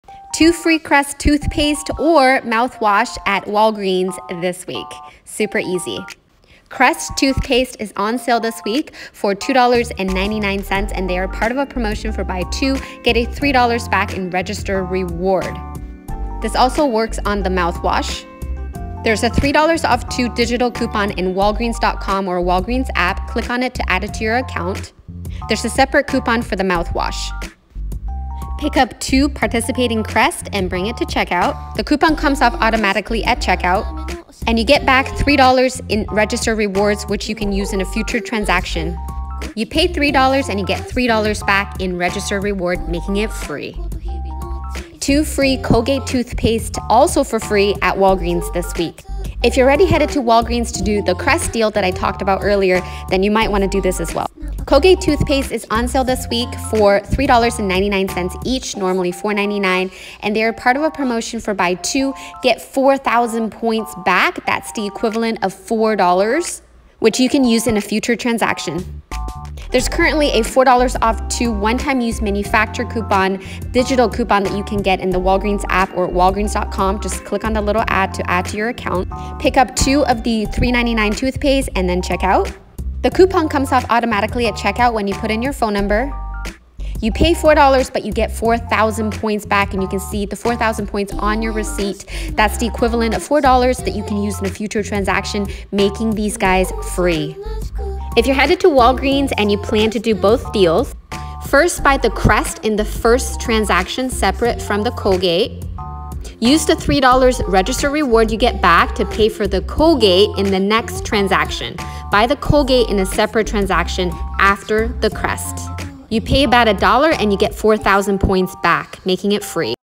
4 EASY Freebies! 🙌 Walgreens Deals Using JUST Your Phone!
2 Different EASY Newbie Friend Freebie Deals Using Just Your Phone at Walgreens this week! In the first couponing deal, score 2 FREE Crest Mouthwash OR Toothpaste. In the second deal, score 2 FREE Colgate.

These are both perfect deals to try if you are new to couponing and want to learn how to coupon. Just sign into your account or create one. At checkout, after you give them your phone number, the coupons come off automatically.

If you want to get fancy and coupon like a ninja. You can roll the rewards like this:

Transaction 1: Buy the Crest deal first. Earn $3 RR.
Transaction 2: Buy the Colgate deal next. Use the $3 RR to Pay for this deal. You spend about $1 and get 4000 points back (like $4 in points).
Final Price: All 4 Items FREE! Just pay tax.